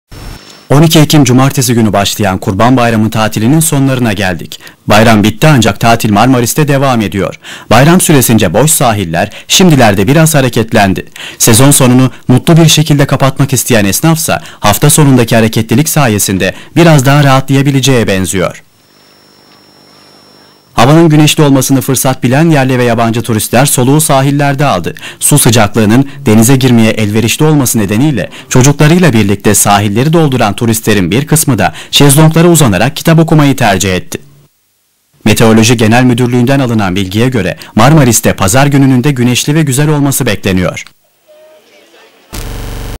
MARMARİS'TE TATİL DEVAM EDİYOR...
HAVANIN GÜNEŞLİ OLMASI VATANDAŞLARI PLAJLARA YÖNELTTİ